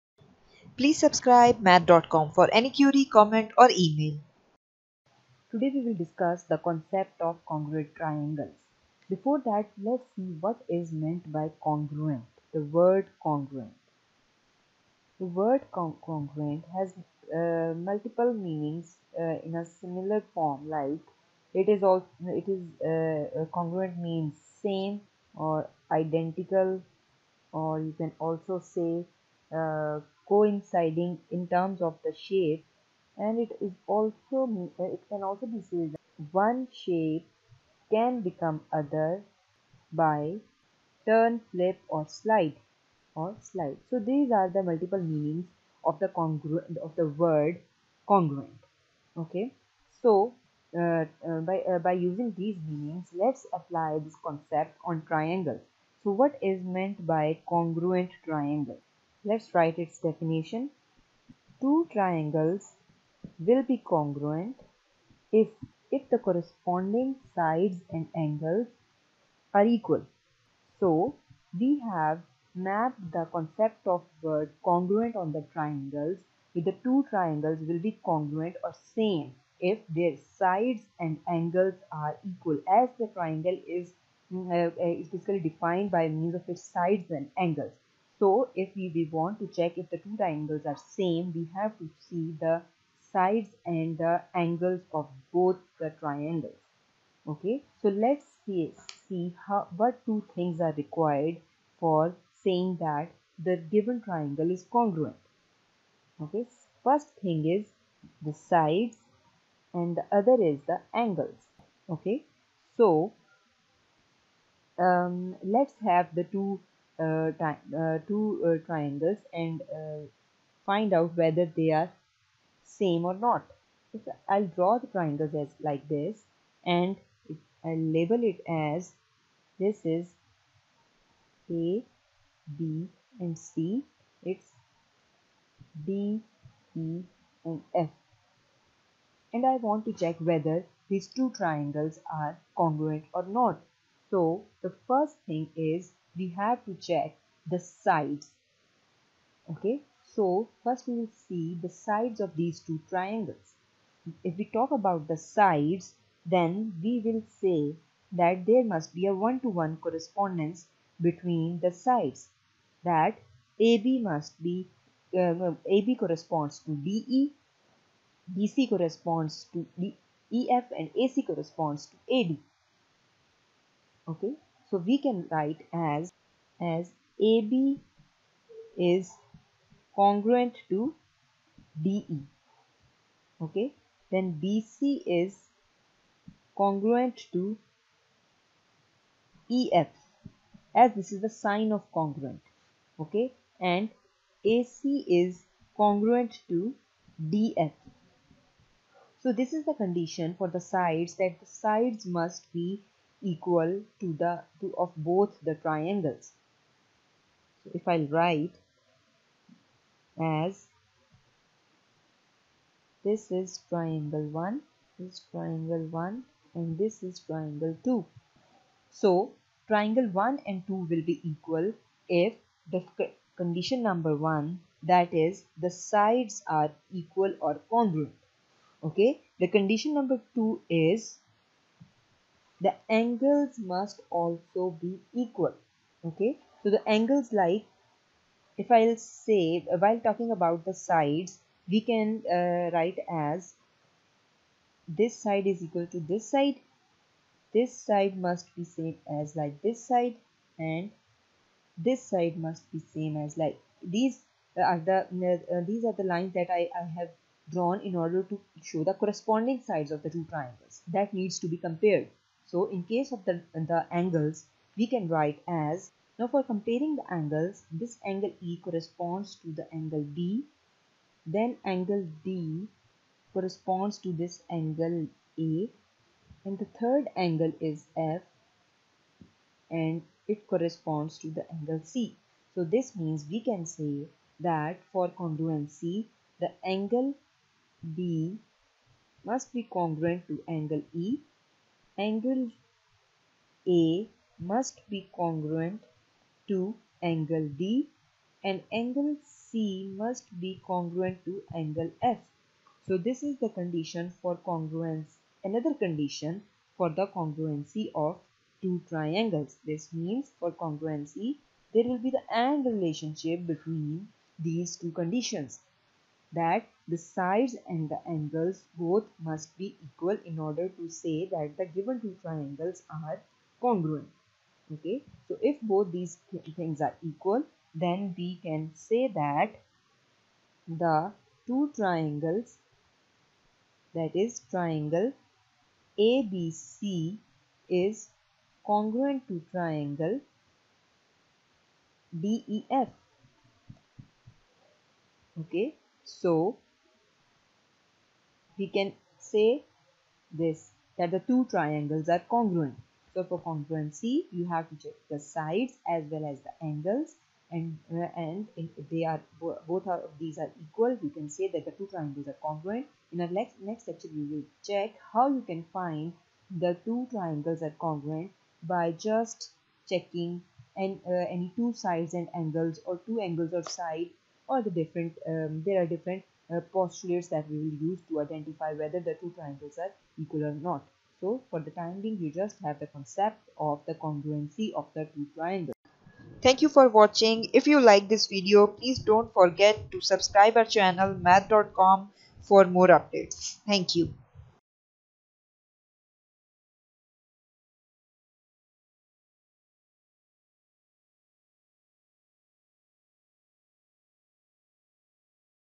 What are Congruent Triangles | Conditions for Triangles Congruence | Math Dot Com
Congruent Triangles. When two triangles are congruent they will have exactly the same three sides and exactly the same three angles. The equal sides and angles may not be in the same position (if there is a turn or a flip), but they are there.So for congruency of two triangles two conditions are must:
1)All the three sides equal of two triangles
2) All the three angles equal of two triangles